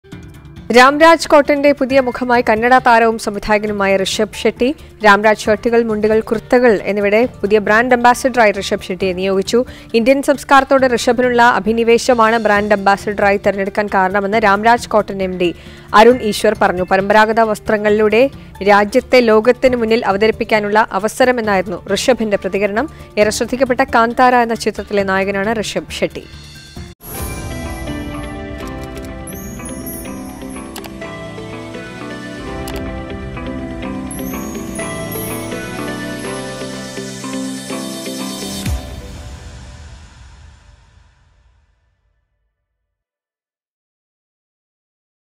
ഋഷഭ് ഷെട്ടി രാംരാജിന്റെ പുതിയ മുഖം | RamRaj | Rishab Shetty
കന്നട താരം ഋഷഭ് ഷെട്ടി രാം രാജ് കോട്ടണിന്റെ പുതിയ ബ്രാൻഡ് അംബാസിഡർ; പരമ്പരാ​ഗത വസ്ത്രങ്ങളോടുള്ള പ്രിയമാണ് ഋഷഭ് ഷെട്ടിയെ അംബാസിഡർ ആക്കാനുള്ള കാരണമെന്ന് രാം രാജ് കോട്ടൺ എംഡി